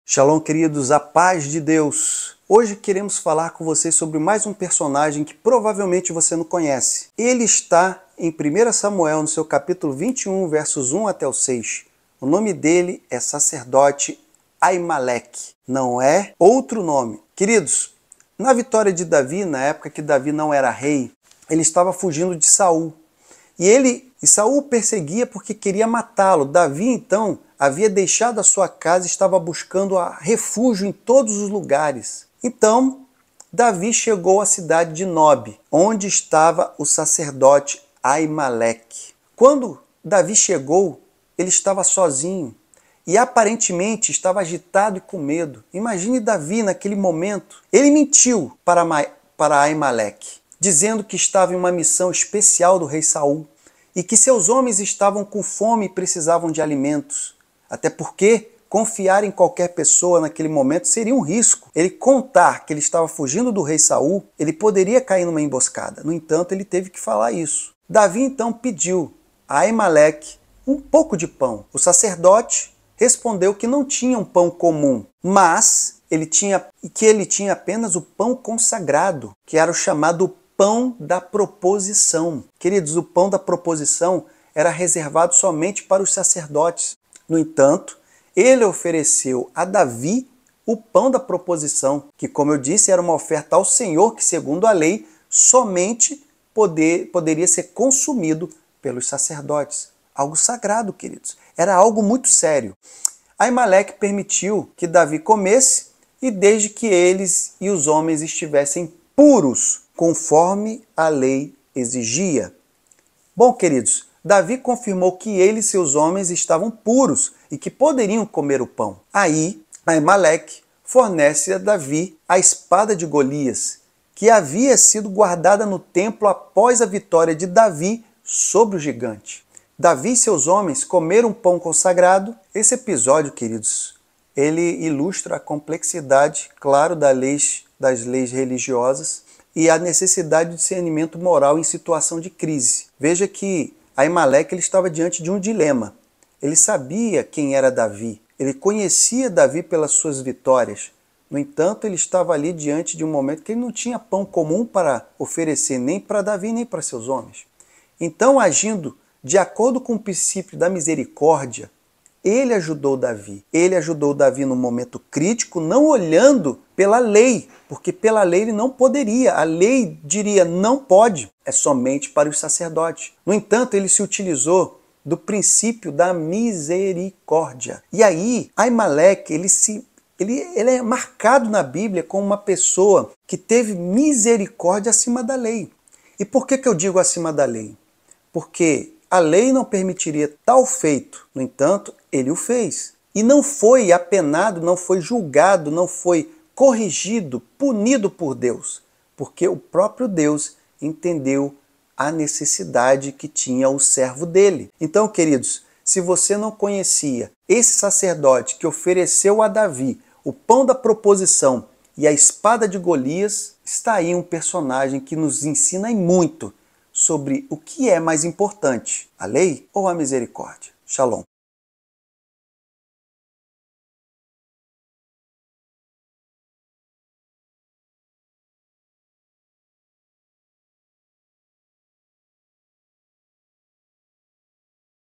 1 SAMUEL 21:01-06 - QUEM FOI O SACERDOTE QUE AJUDOU DAVI?
1 SAMUEL 21:01-06 -  QUEM FOI O SACERDOTE QUE AJUDOU DAVI?

A Vida de Davi: Na época, Davi ainda não era rei e estava fugindo de Saul, que o perseguia com a intenção de matá-lo. Davi havia deixado sua casa e estava buscando refúgio em diferentes lugares.

Verso 1: Davi chegou à cidade de Nobe, onde estava o sacerdote Aimeleque. Quando chegou, Davi estava sozinho e aparentemente estava agitado e com medo. Ele mentiu para Aimeleque, dizendo que estava em uma missão especial do rei Saul e que seus homens estavam com fome e precisavam de alimentos.

Versos 2-3: Davi pediu a Aimeleque um pouco de pão. O sacerdote respondeu que não tinha pão comum, apenas pão consagrado, que era reservado para os sacerdotes. No entanto, ele ofereceu a Davi o pão da proposição, que era uma oferta ao Senhor e que, segundo a lei, só podia ser comido pelos sacerdotes. Aimeleque permitiu que Davi comesse esse pão, desde que ele e seus homens estivessem puros, conforme a lei exigia.

Versos 4-5: Davi confirmou que ele e seus homens estavam "puros" e poderiam comer o pão. Aimeleque também forneceu a Davi a espada de Golias, que havia sido guardada no templo após a vitória de Davi sobre o gigante.

Verso 6: Davi e seus homens comeram o pão consagrado, e a presença da espada de Golias simbolizava o poder de Deus e a vitória sobre os inimigos.

🙏 Cursos que eu indico para o seu conhecimento: 👨‍🎓

💥Para nos enviar  um presente:
CAIXA POSTAL
119331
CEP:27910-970